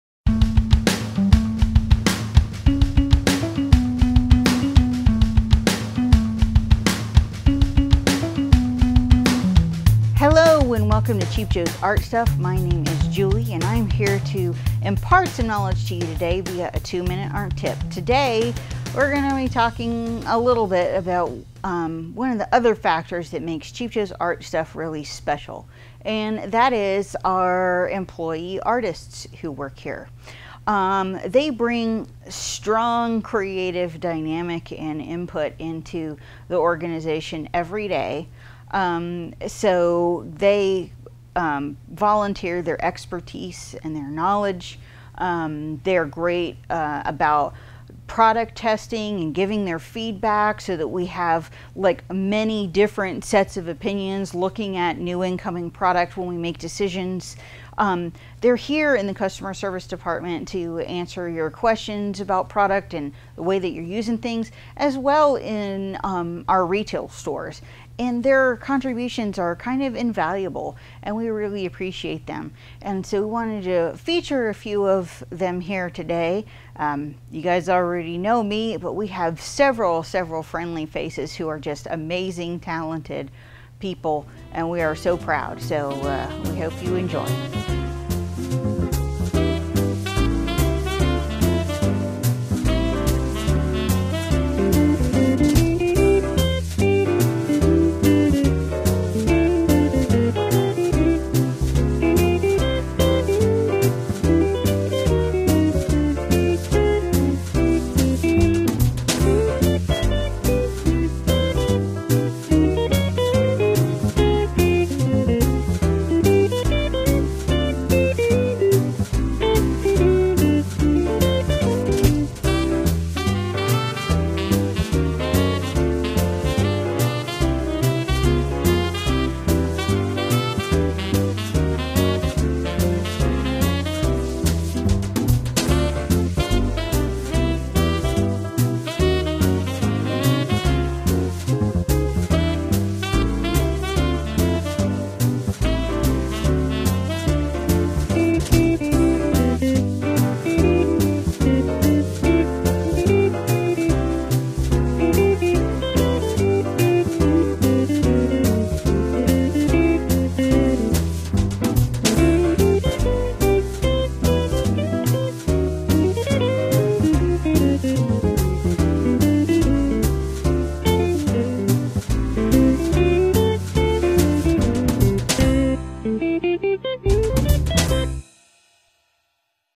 Cheap Joe's 2 Minute Art Tips - What Makes C J 's Special? Employee Artists
So many Cheap Joe's Art Stuff employees are also fantastic artists. In this edition of 2 Minute Art Tips, Julie will introduce you to six of those talented folks.

Make more art and spend less money by shopping at Cheap Joe's Art Stuff.